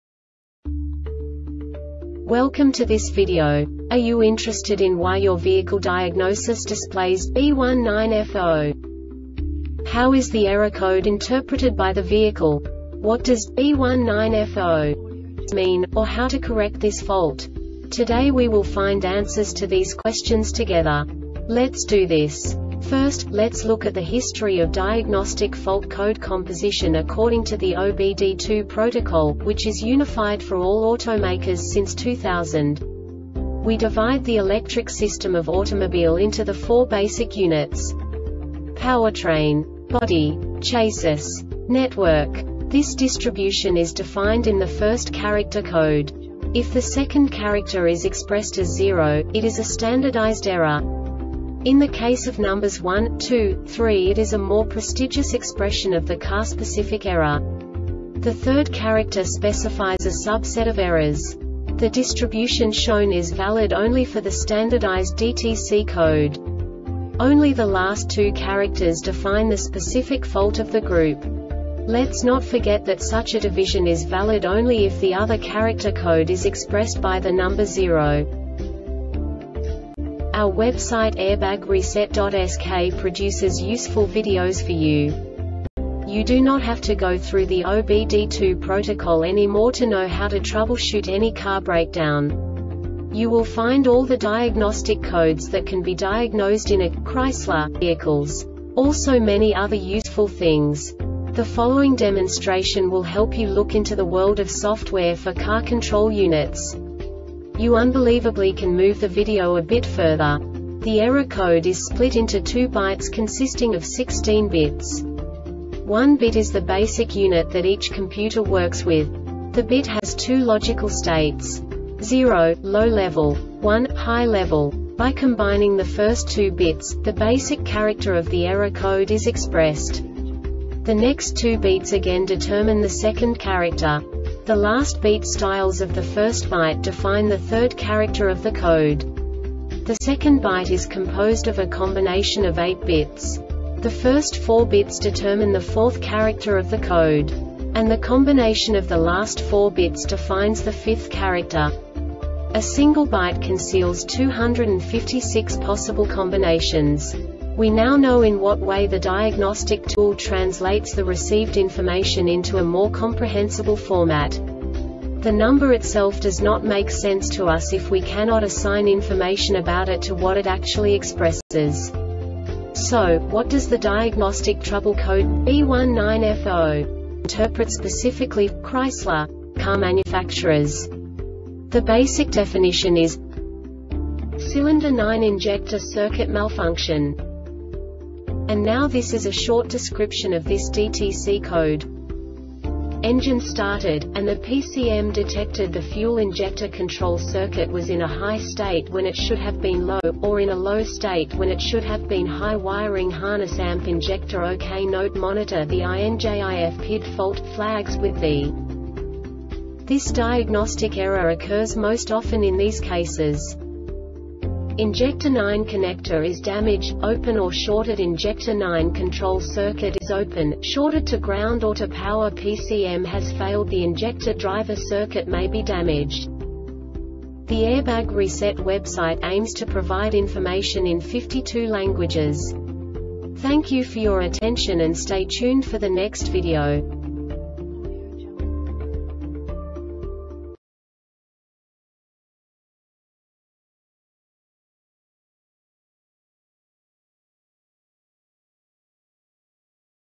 DTC Chrysler B19F0 Short Explanation
Contents:
0:21 Basic DTC analysis according to OBD2 protocol standard.
1:48 Insight into programming
2:58 A diagnostically understandable description of the Diagnostic Trouble Code
3:05 Basic error definition

Make:
Chrysler
Code:
B19F0
Definition:
Cylinder 9 Injector Circuit Malfunction
Description:
Engine started,and the PCM detected the fuel injector control circuit was in a high state when it should have been low,or in a low state when it should have been high (wiring harness &amp;injector okay). Note: Monitor the INJIF PID Fault "flags" with the
Cause:
Injector 9 connector is damaged,open or shortedInjector 9 control circuit is open,shorted to ground or to powerPCM has failed (the injector driver circuit may be damaged)